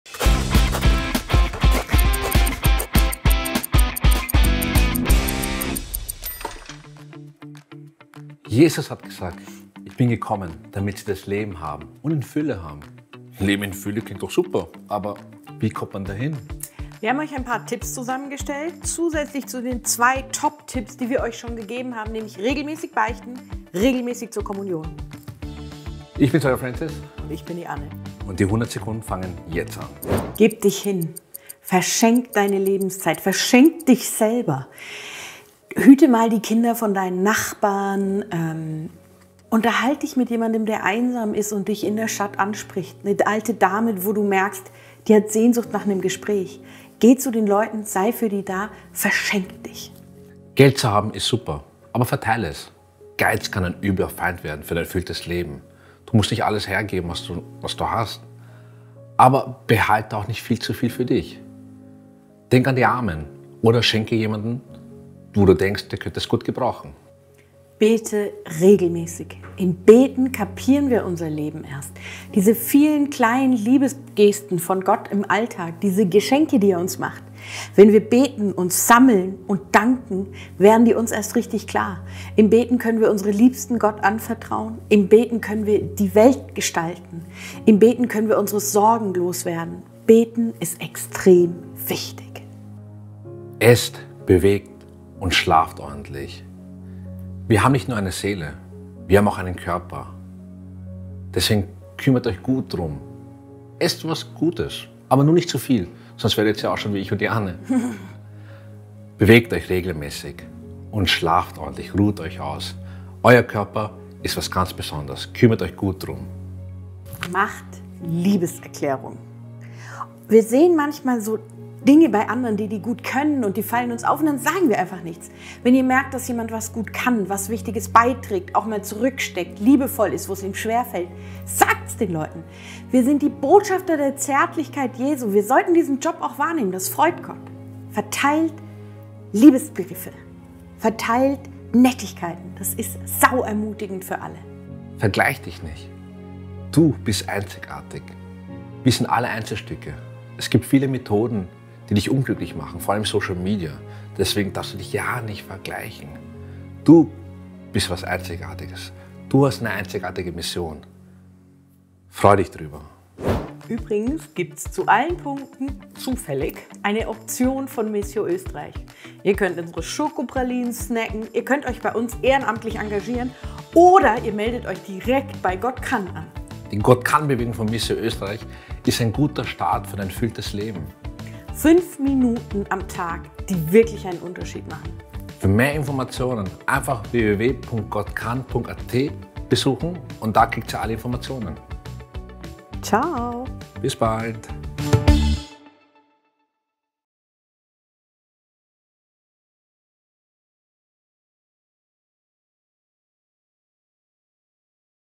In 100 Sekunden Episode 3: LEBEN IN FÜLLE
Jesus hat uns Menschen ein Leben in Fülle angekündigt. Klingt gut. Aber wie geht das eigentlich? Wir haben sechs gut umsetzbare Tipps zusammengestellt. Was sind eure Tipps? Hinterlasst einen Kommentar!